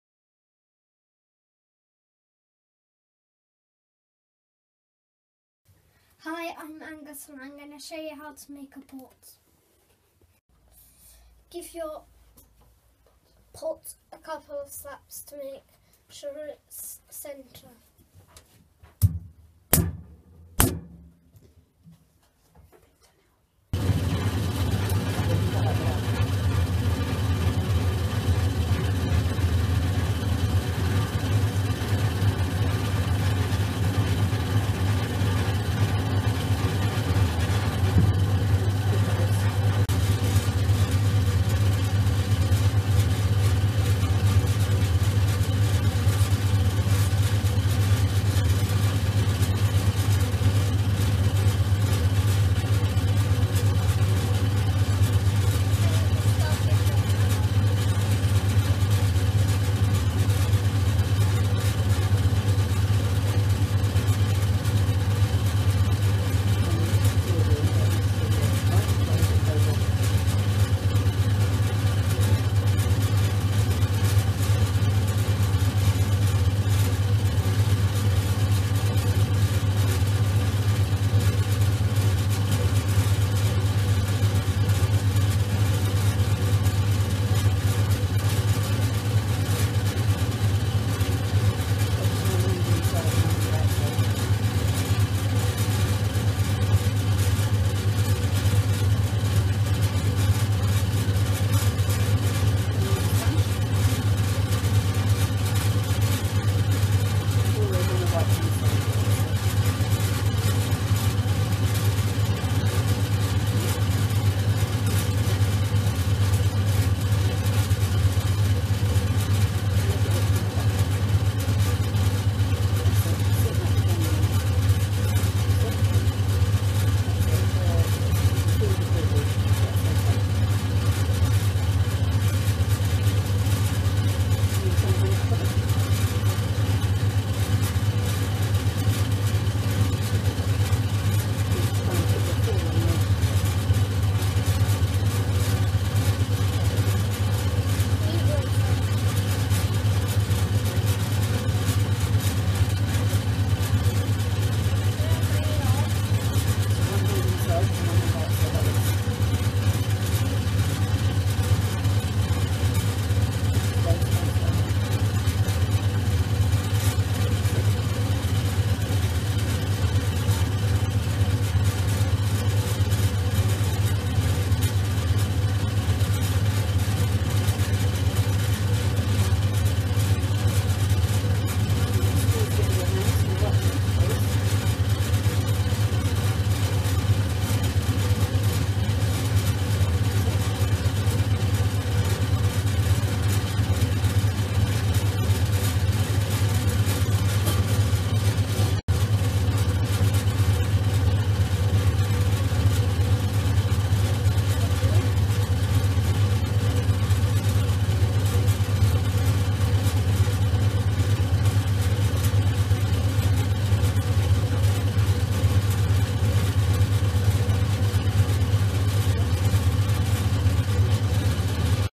The Asparagus Collection by Malthouse Ceramics
6 year old introduces himself before he makes a pot